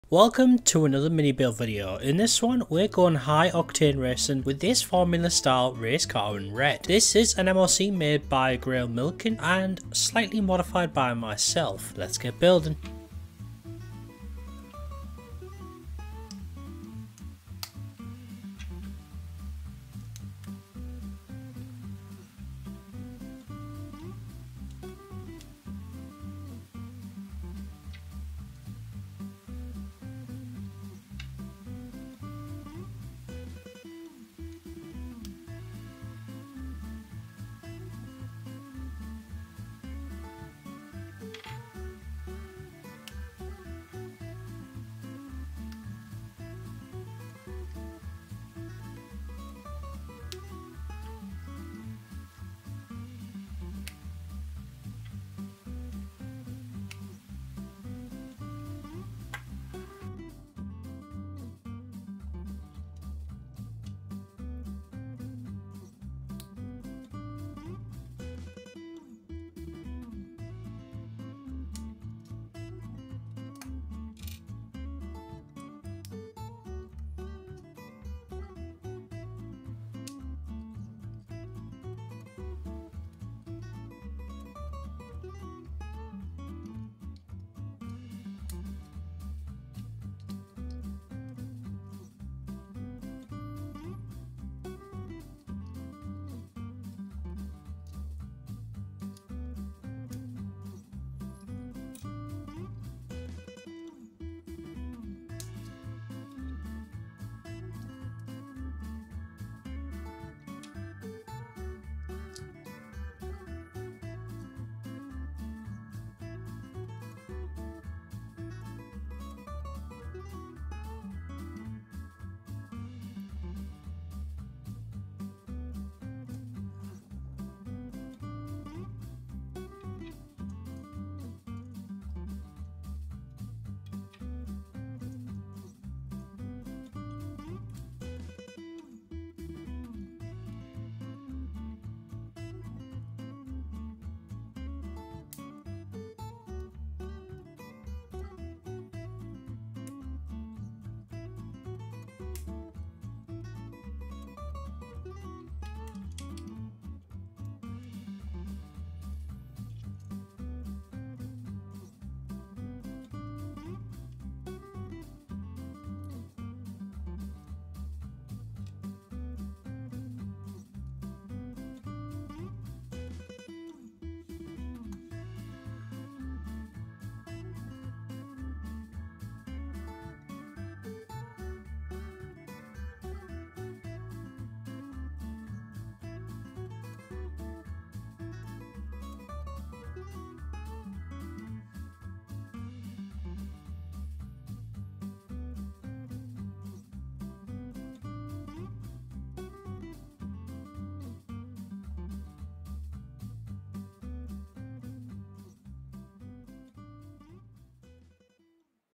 Formula Race Car - Simple & Quick LEGO Build Tutorial
In this mini build video, we're going high-octane racing with our very own formula-style race car! What do you think of it?

Latest Lego Deals